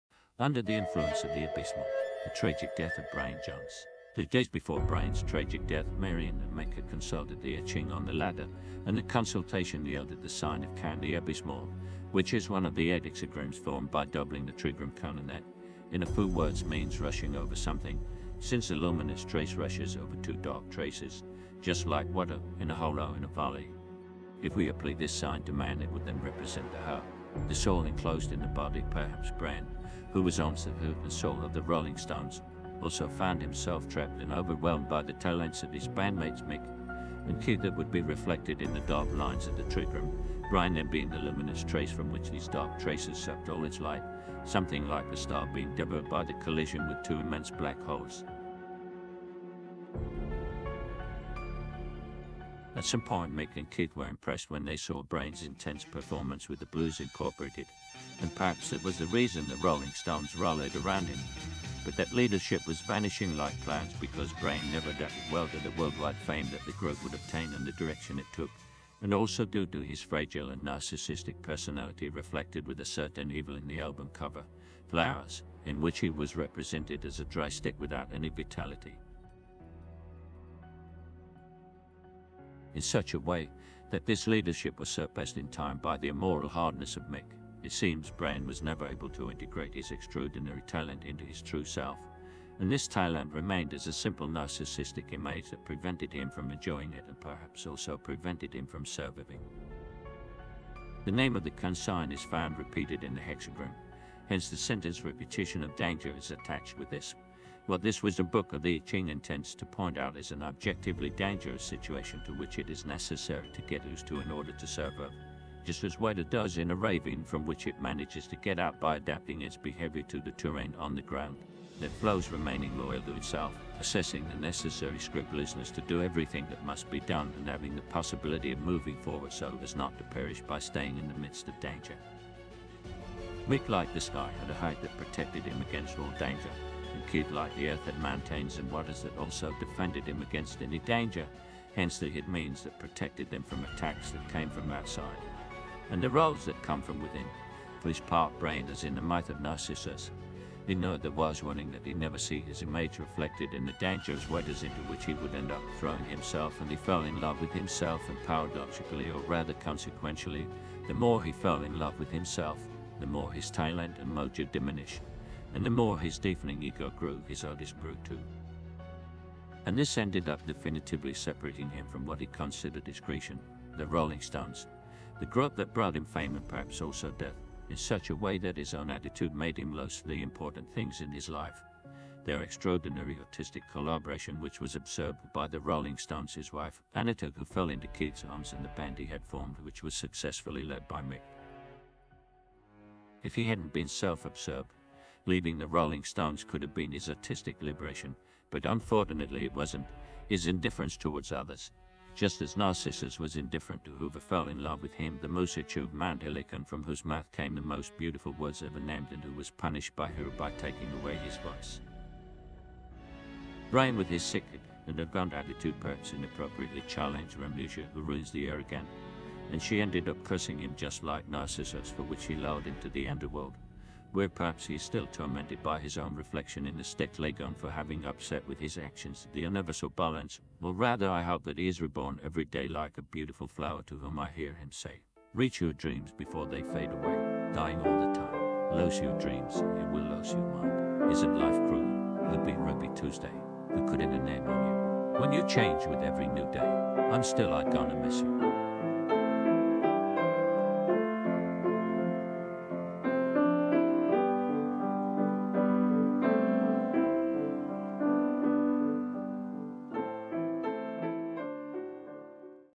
Under The Sign Of Abysm The Tragic Death Of Brian Jones
Brian Jones, the founder father of the Rolling Stones, is a mythological god of Rock & Roll, Like Narcissus, he lost his life for falling in love with himself that led to suicidal autism. His death is still surrounded by mystery and suspicion.